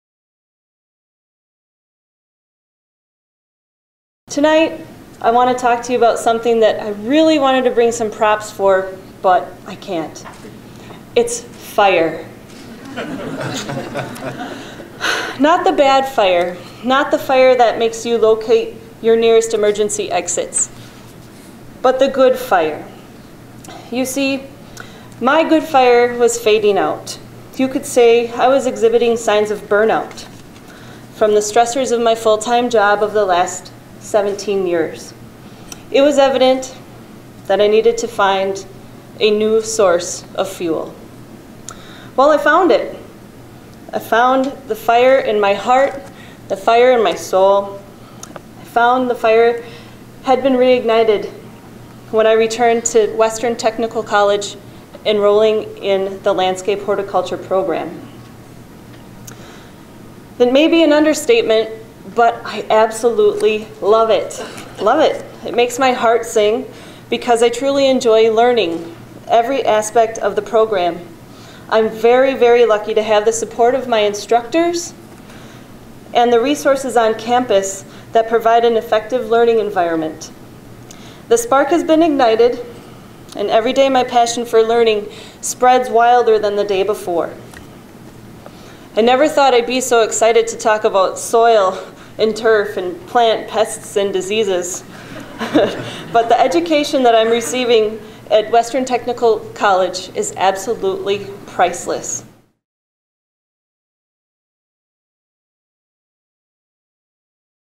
Western Sarah speech
This Student Ambassador was experiencing massive burnout when she decided to return to college for a new career. She is now excited about what she's learning and loves the opportunity to put it into practice.